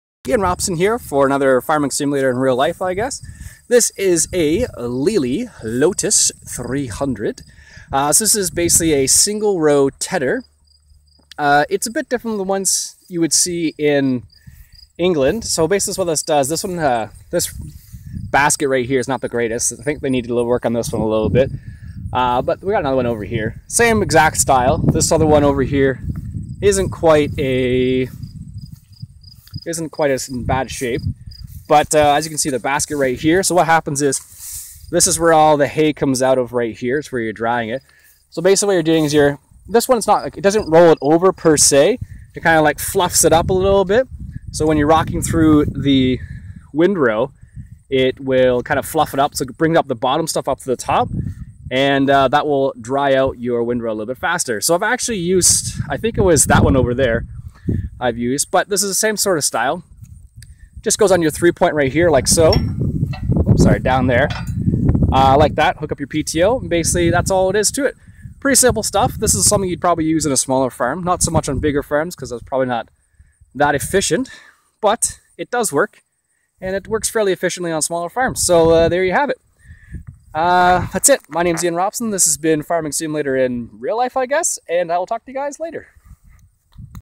Farming Simulator IRL: Lely Lotus 300 Tedder
In this video you see what a single row tedder looks likes. License for let's plays - Farming Simulator 15 or 13